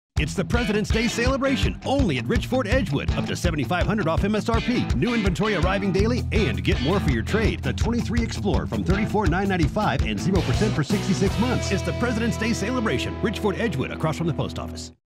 Rich Ford Edgewood 2024 Presidents Day #2
It’s the Presidents’ Day celebration, only at Rich Ford Edgewood!
Up to $7,500 off MSRP, new inventory arriving daily, and get more for your trade!
Get the 2023 Ford Explorer for $34,995!
It’s the Presidents’ Day celebration at Rich Ford Edgewood, across from the Post Office.